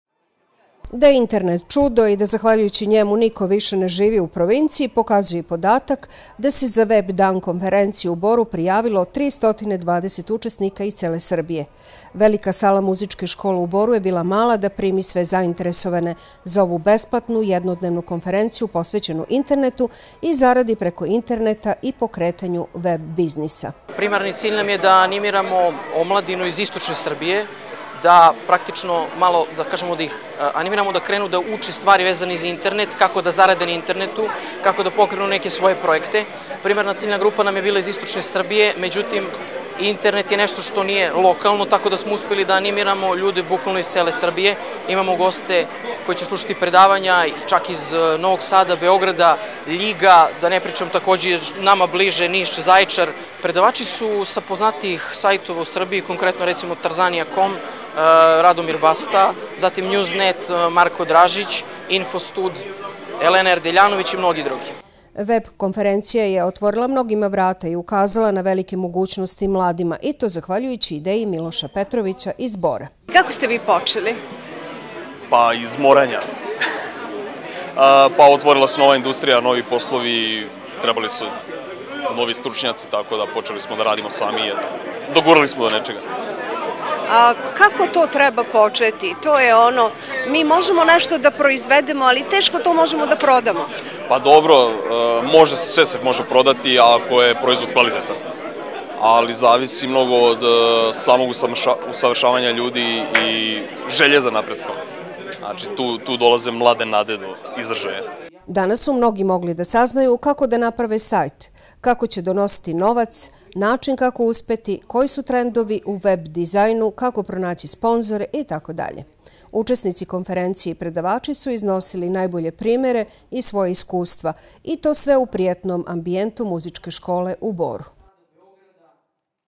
WebDan 2013 - Reportaža TV Bor
Reportaža TV Bor sa prve WebDan konferencije, 13. april 2013.